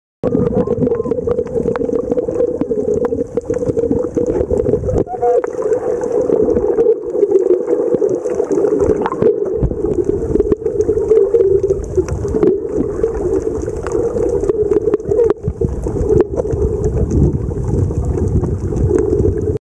manatee carrie bow caye
Manatee sighting on shallow reefs at Carrie Bow Caye, Belize , while doing surveys for Stony Coral Tissue Loss Disease